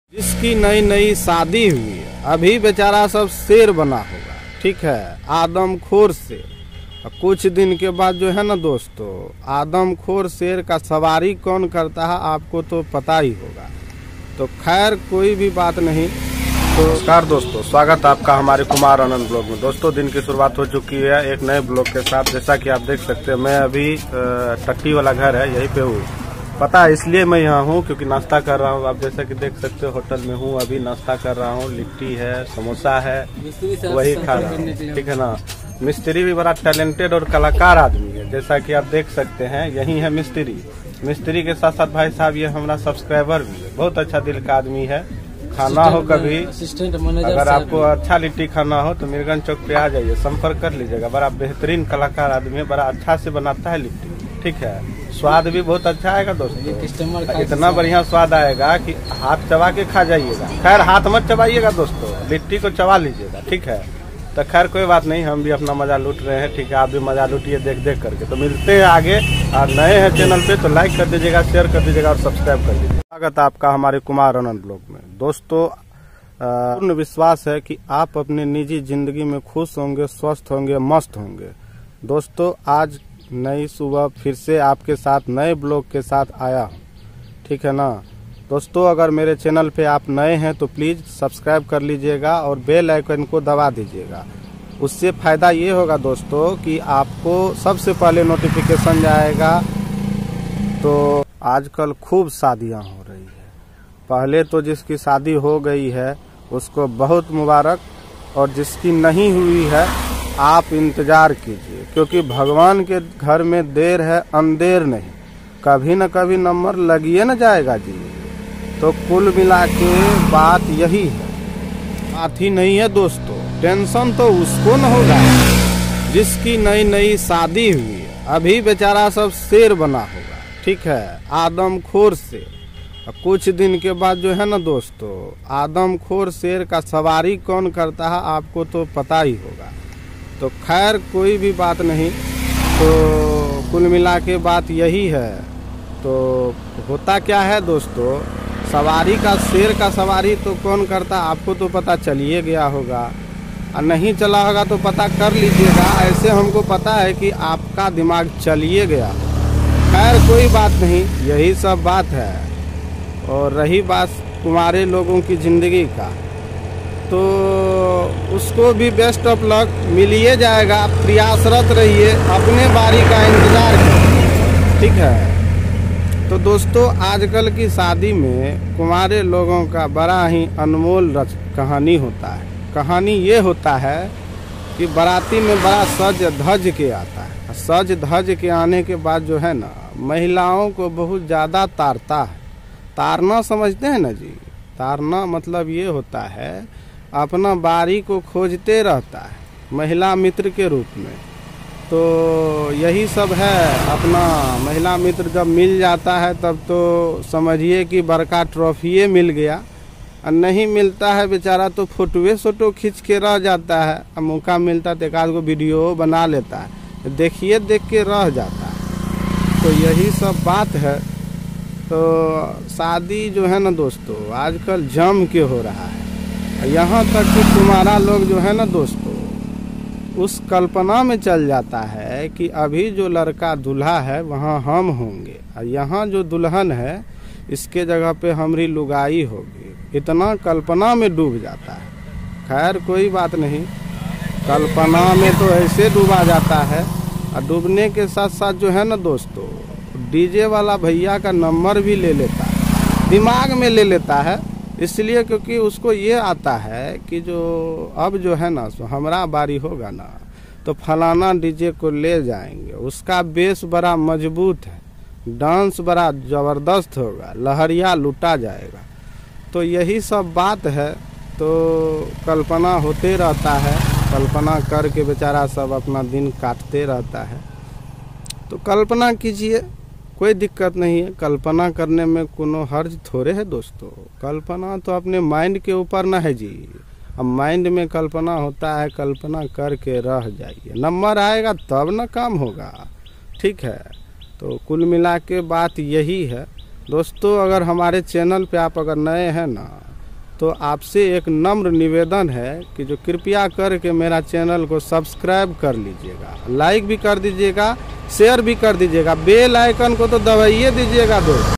Kunwaro Ki Jindgi || Shadi wala Funny Vlog||Kumar Anand Vlog
Kunwaro Ki Jindgi || Shadi wala Funny Vlog || KumarAnand Vlog||
नमस्कार दोस्तो आज के व्लोग में आपलोग को कुँवारे लोगों की जिंदगी के बारे मे दिखाया उम्मिद करता हु की अच्छा लगा होगा